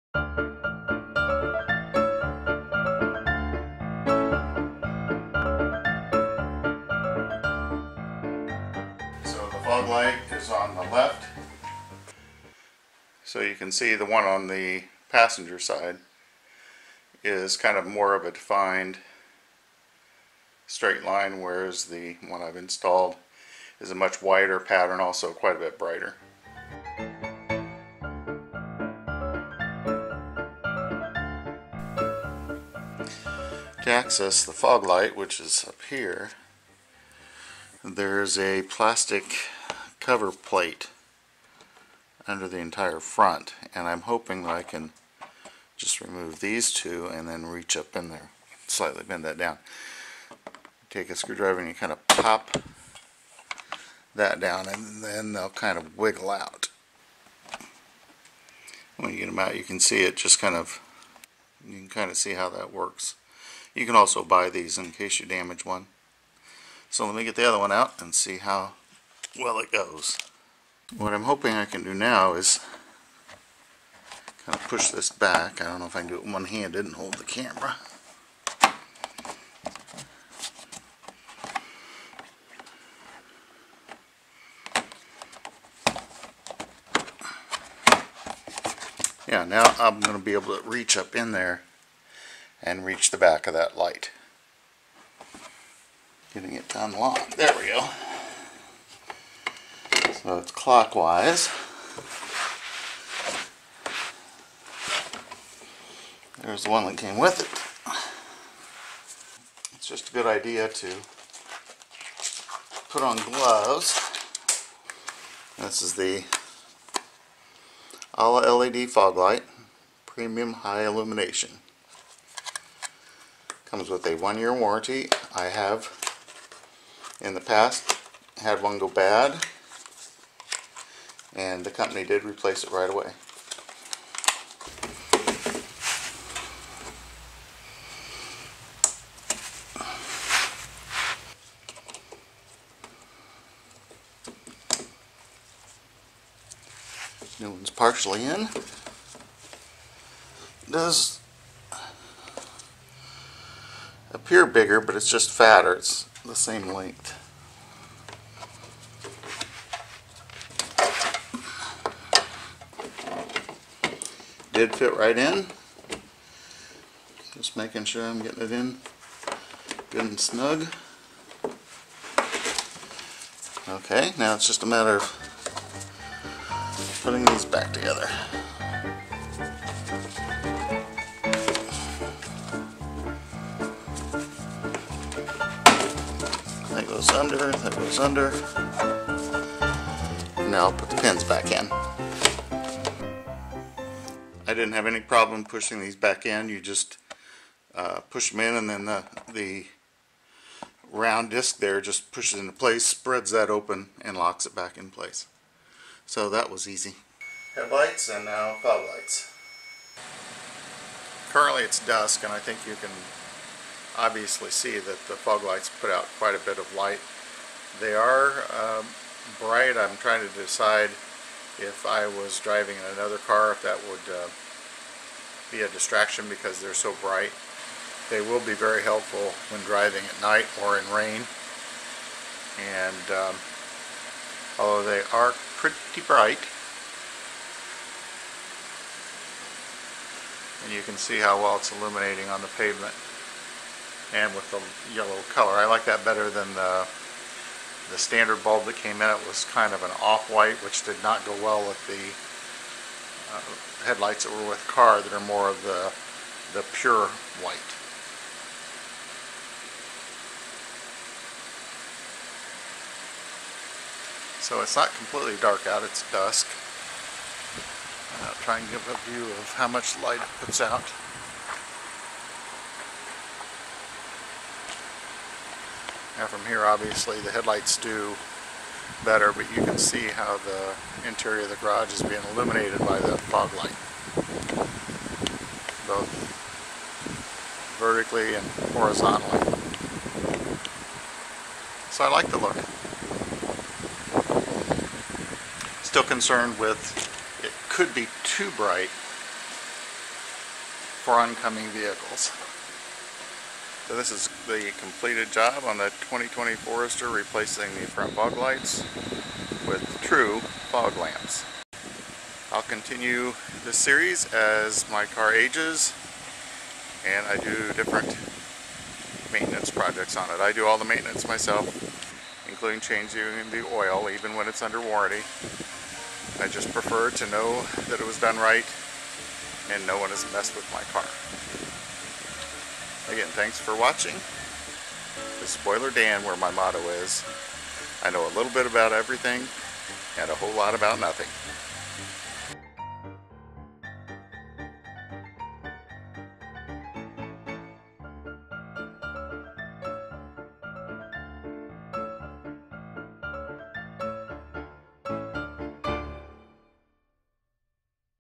Alla Lighting LED (Fog / DL) Bulb 3000K Amber Yellow: Review and Installation
Installation on Subaru Forester 2020 model year.